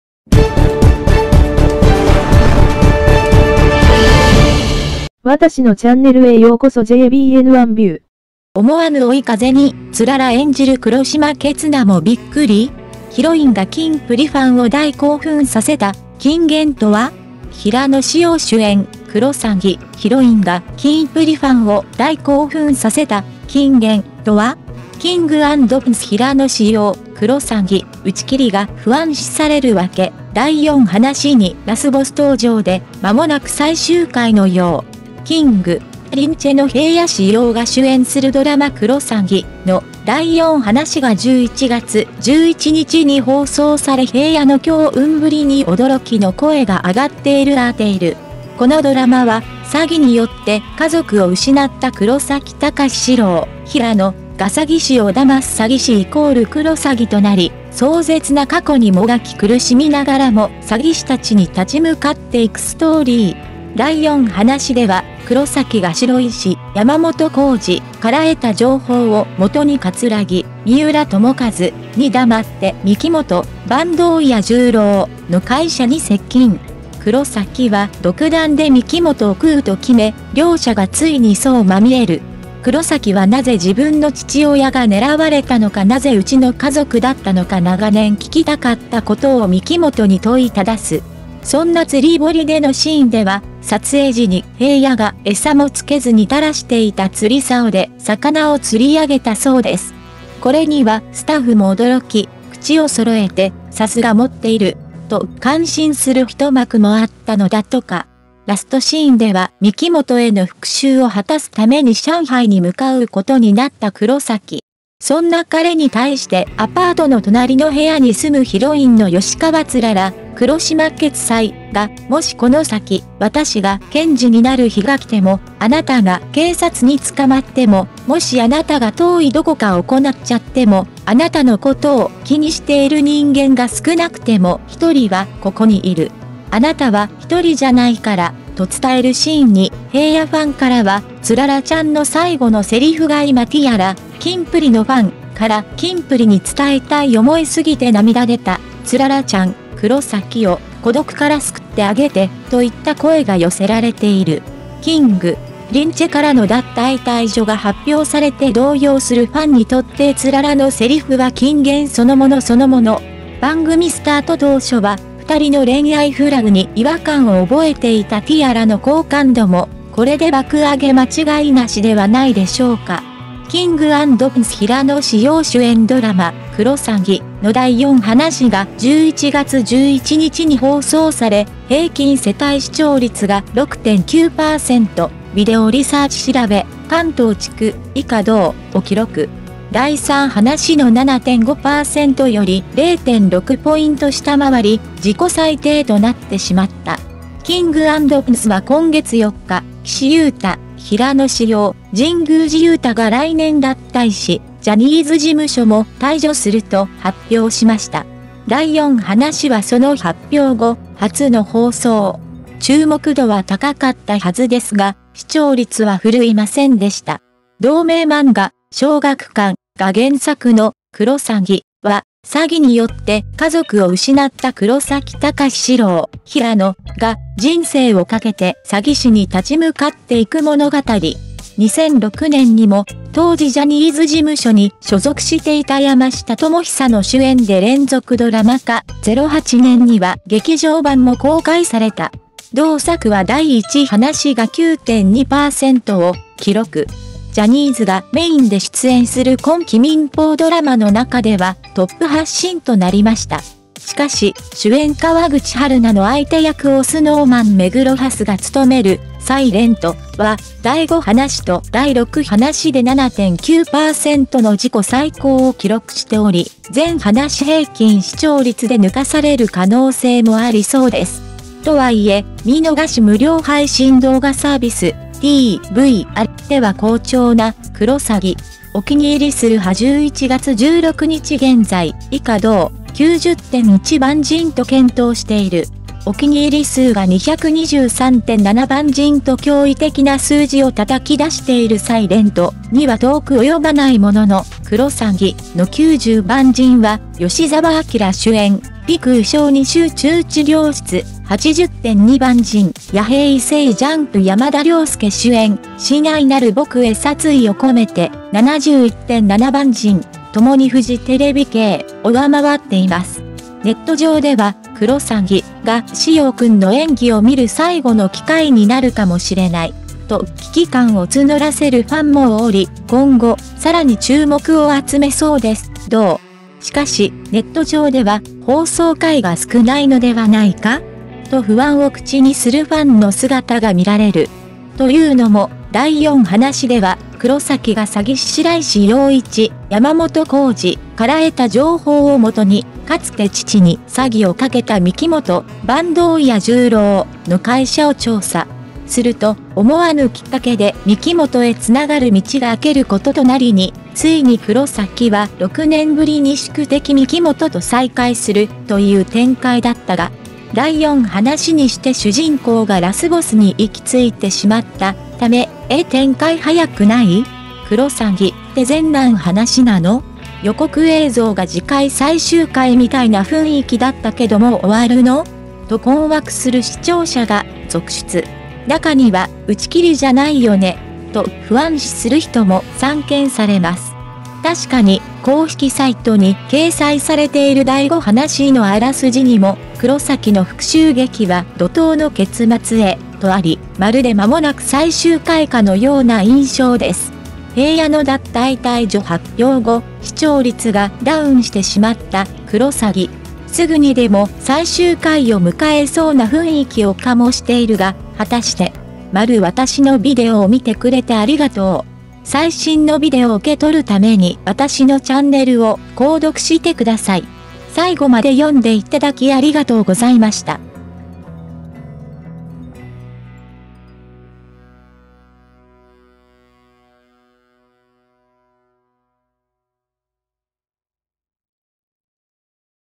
【平野紫耀】ヒロインがキンプリファンを大興奮させた「金言」とは？。打ち切りが不安視されるワケ
《動画の内容》
King & Prince Hirano Sho's "Kurosagi", the reason why people are worried about the cancellation - "It looks like the final episode soon" with the appearance of the final boss in episode 4
The 4th episode of the drama "Kurosagi" starring King & Prince's Sho Hirano will be broadcast on November 11th, and there are voices of surprise at Hirano's strong carrying.

イケメン # クロサギ# ジャニーズ# ファン # 平野紫耀 # 黒島結菜 Snow Man#King＆Prince#目黒蓮